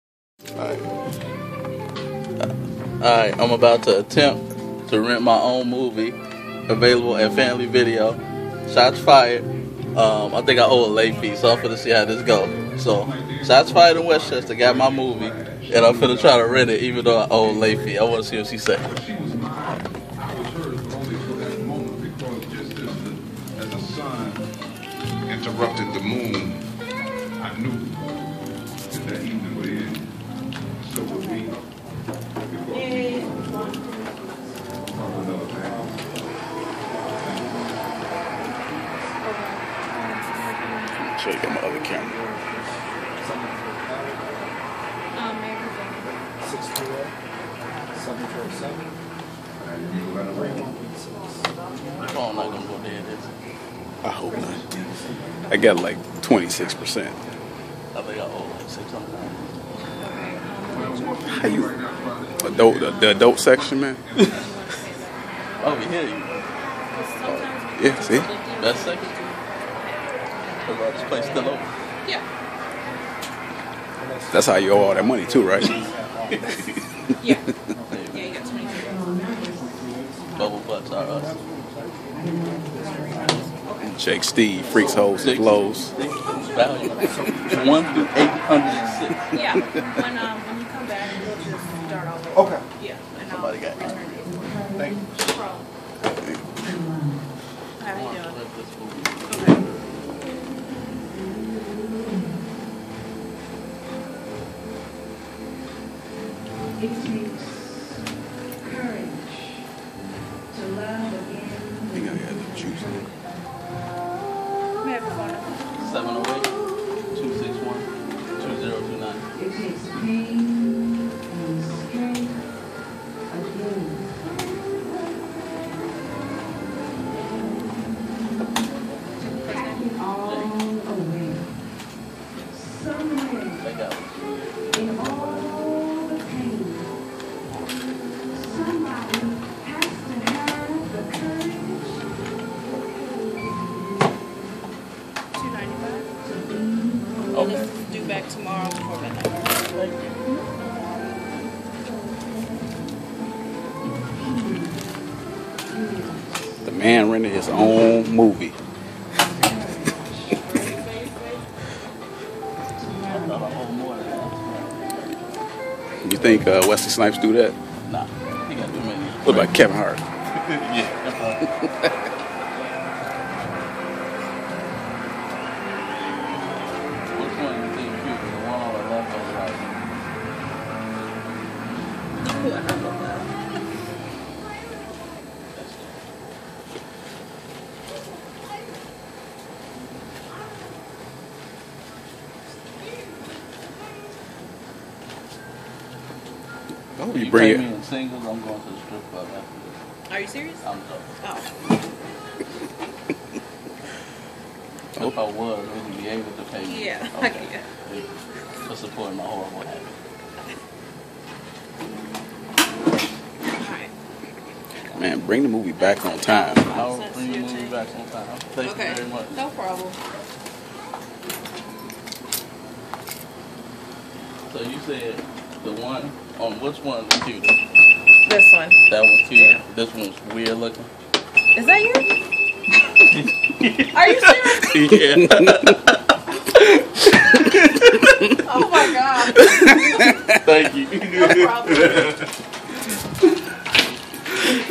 Jay Davis Rents a copy of his own movie from Family Video
On Tuesday September 22nd.  Shots Fired was released globally by Maverick Entertainment.  This video was shot by Donte "Diesel" Williams at Family Video in Westchester IL